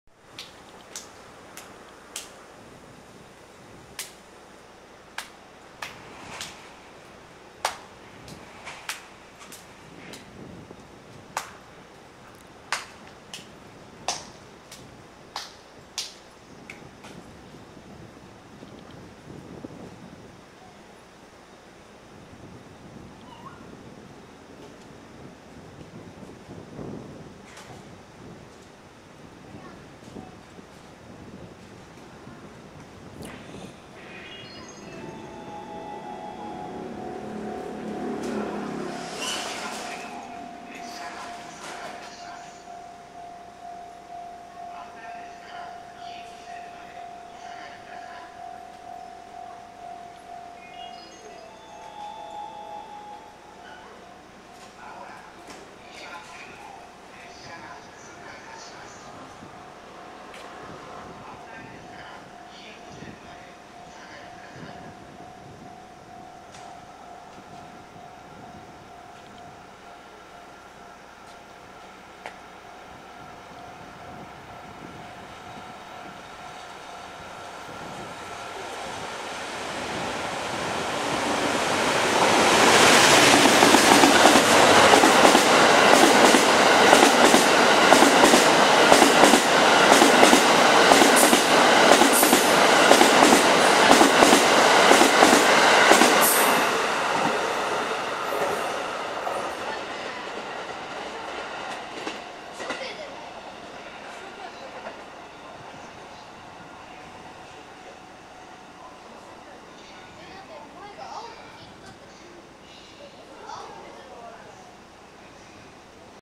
東北本線蓮田駅 寝台特急カシオペア通過光景
ホームの上野寄りには沢山のカメラマンがいたので、仙台寄りでの撮影。